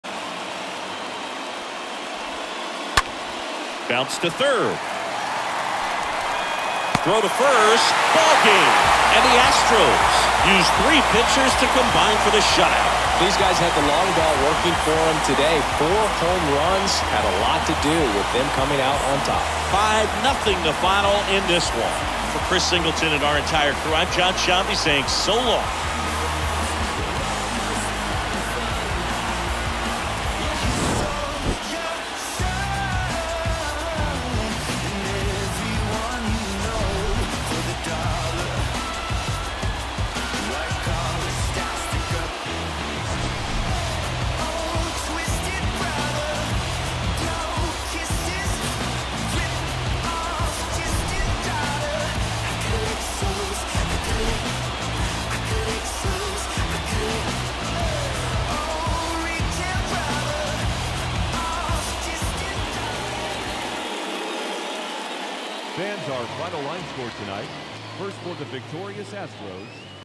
(15-7) SEA @ (25-3) HOU 7/18/22
McCullers Masters Mariners
Mariners 0 - Astros 5
POG: Lance McCullers Jr.